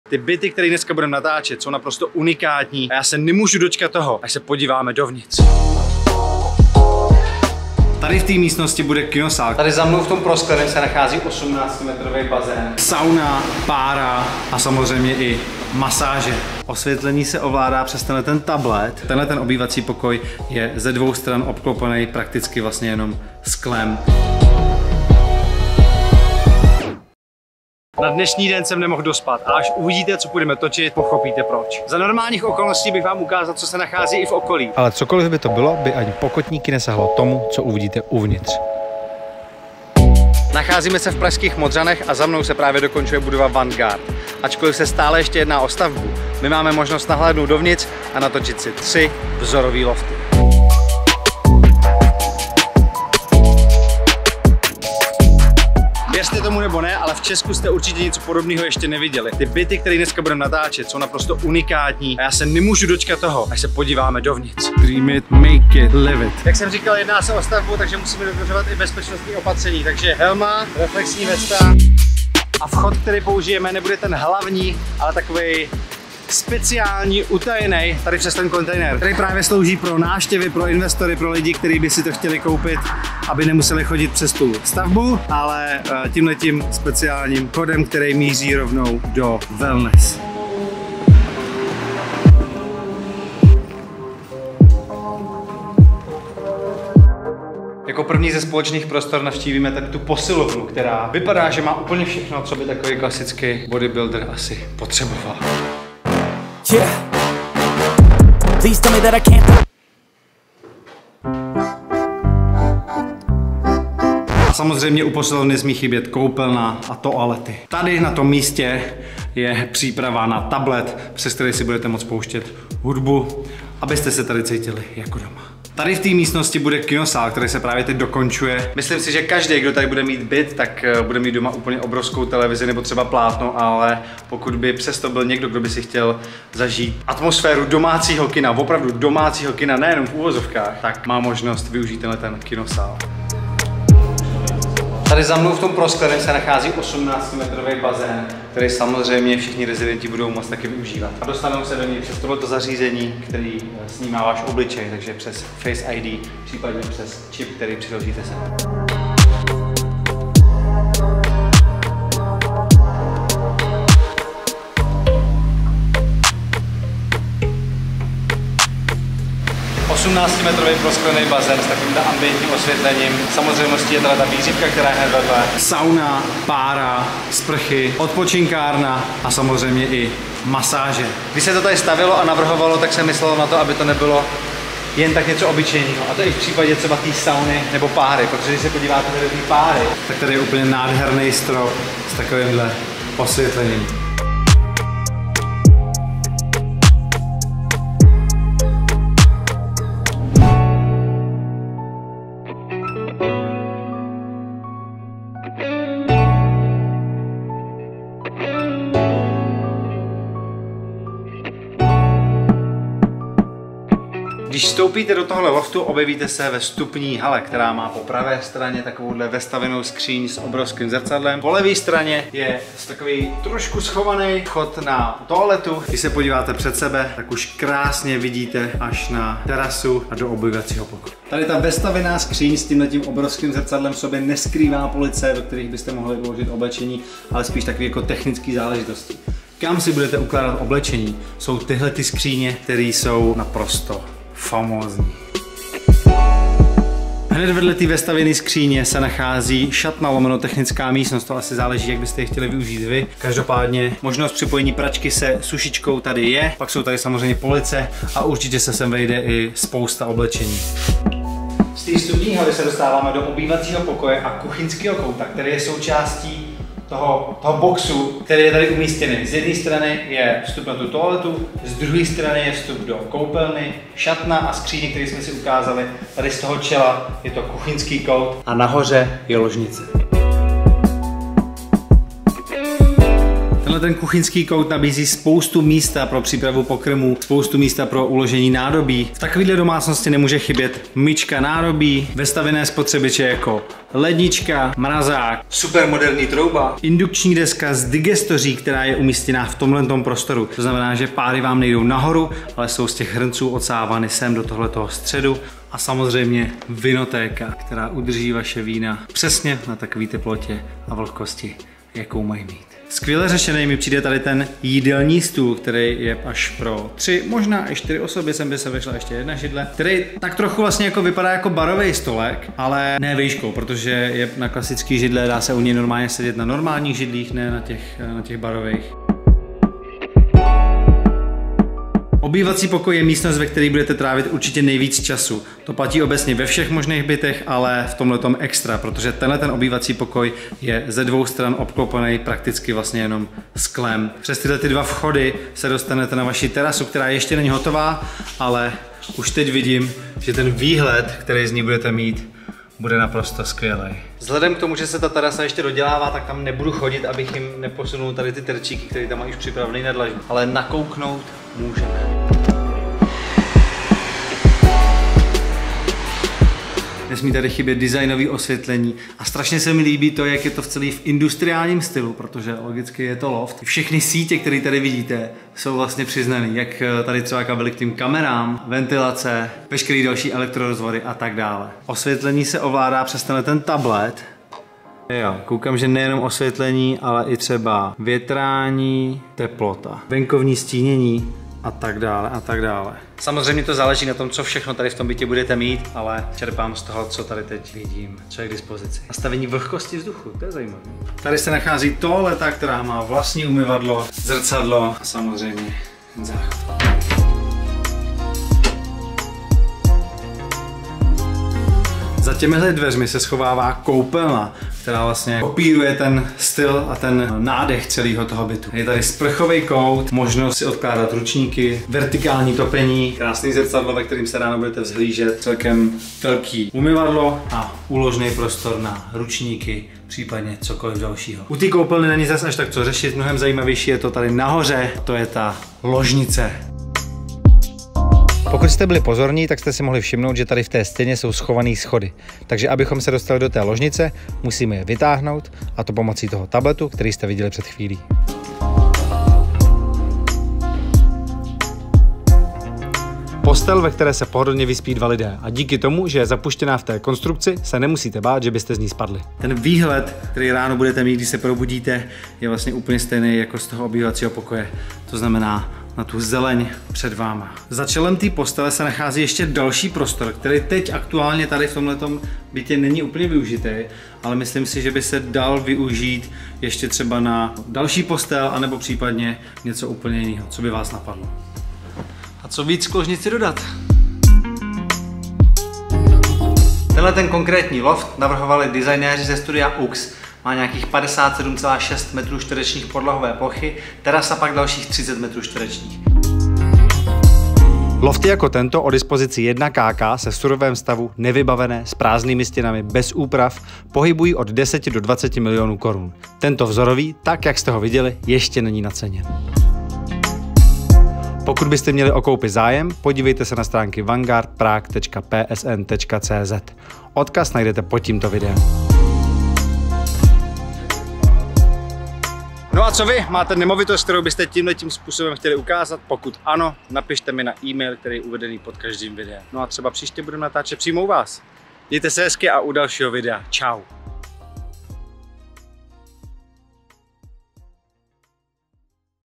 Stylové bydlení v loftu s bazénem, saunou, párou a dalšími wellness | Vanguard Prague
🏠 Máte zájem o koupi? Podívejte se na: vanguardprague.psn.cz

⚪️ Vanguard patří k těm nejzajímavějším projektům v Praze. Bývalá tovární budova se mění na luxusní lofty, které vynikají nejen obrovskými prostory, ale i unikátním wellness, které dům nabízí. Noví majitelé se tak mohou těšit na 18m bazén, vířivku, saunu, páru, masáže, terasu a další. Vzorový byt navrhovalo studio OOOOX (@TheWorldofOOOOX) a ačkoliv se jedná pouze o 1+kk, je naprosto famózní.

✌🏻 Poděkování za umožnění natáčení v nemovitosti: PSN.cz